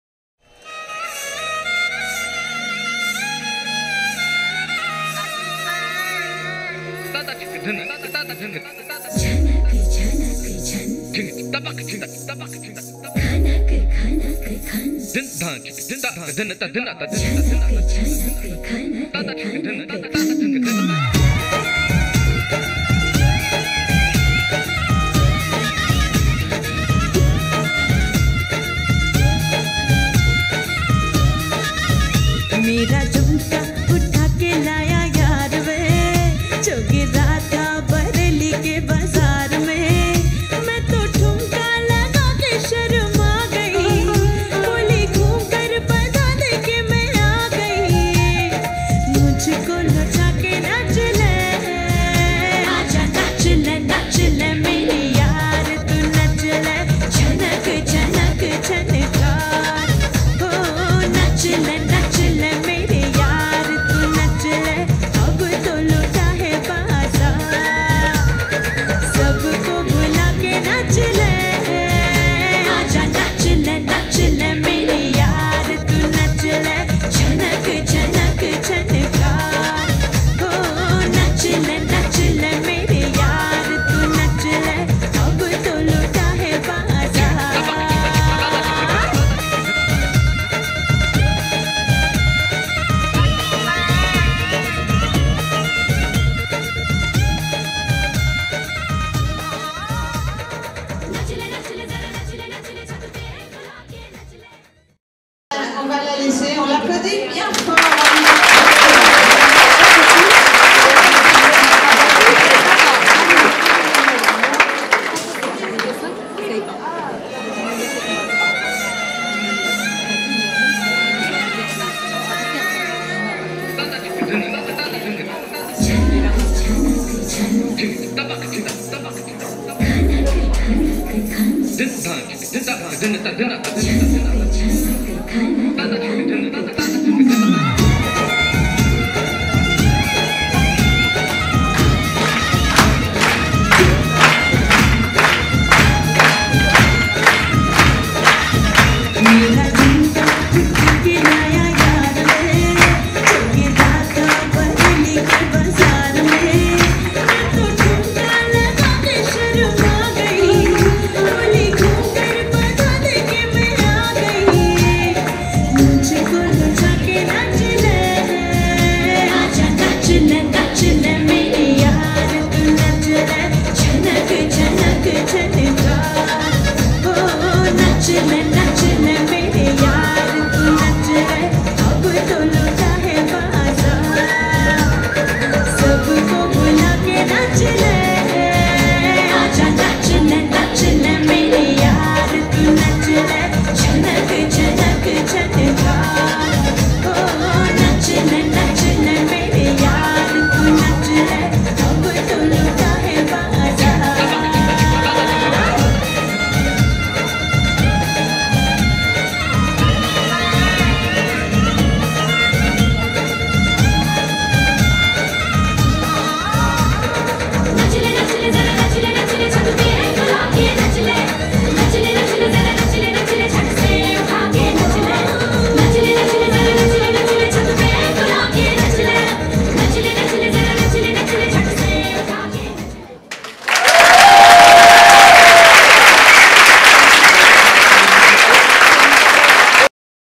FETE MONDIALE DE LA DANSE 2023 BOLLYWOOD
Fête Mondiale de la Danse du 22 Avril 2023 au Barp
Démonstration de Bollywood
Bravo Saranya tu as été magnifique, rendez vous l'an prochain !